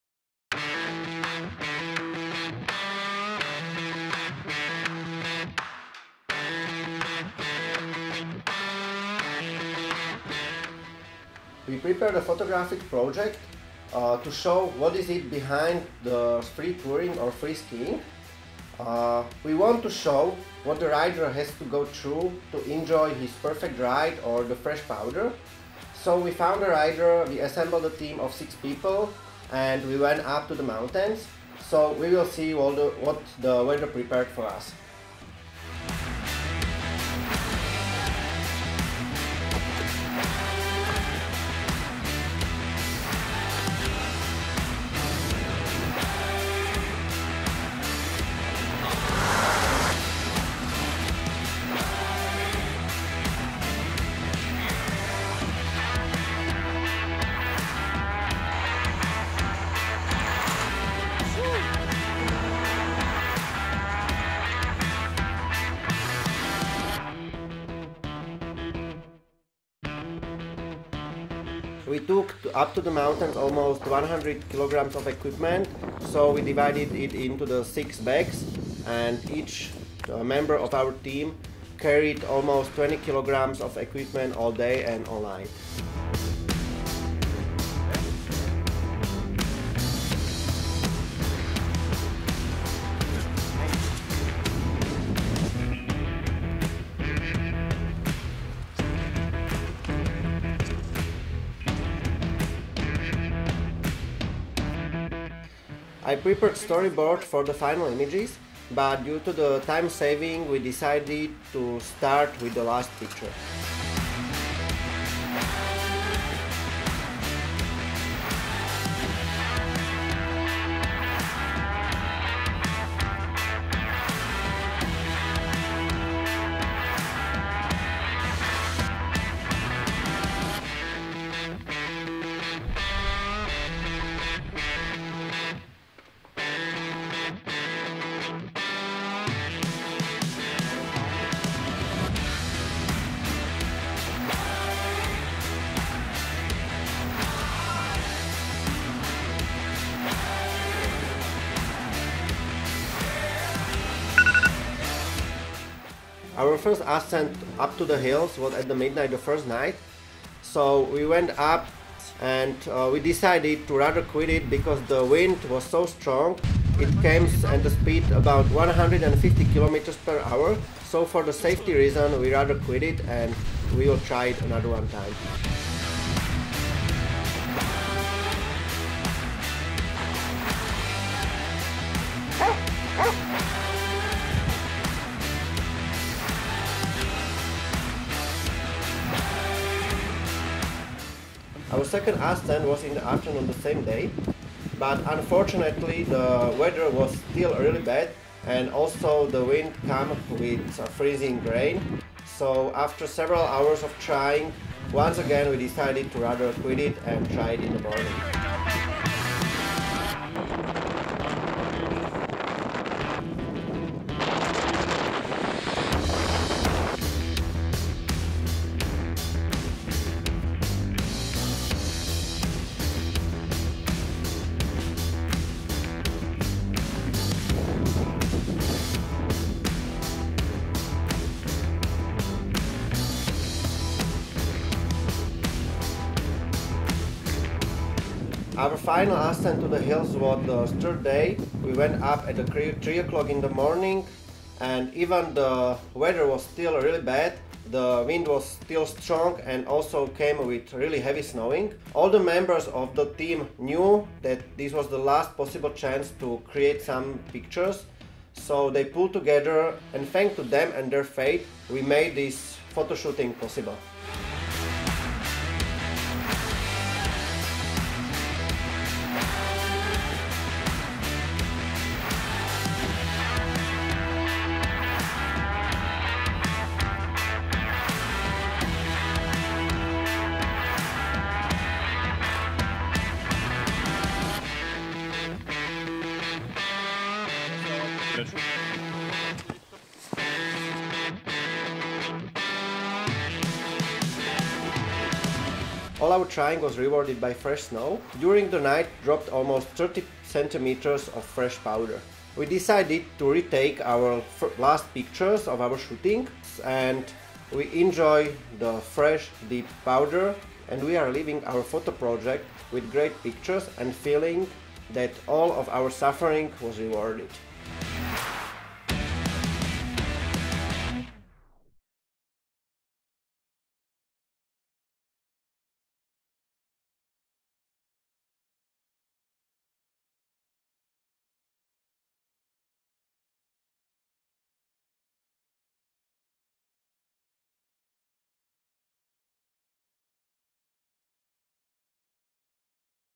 What it takes to create your own story
Fotografický projekt Petra Brichtu, ktorý ukázal, aké neuveriteľne náročné dokáže byť fotenie zimných športov v extrémnych podmienkach. Viac Peťových projektov, recenzií a dobrých rád nájdete v našom magazíne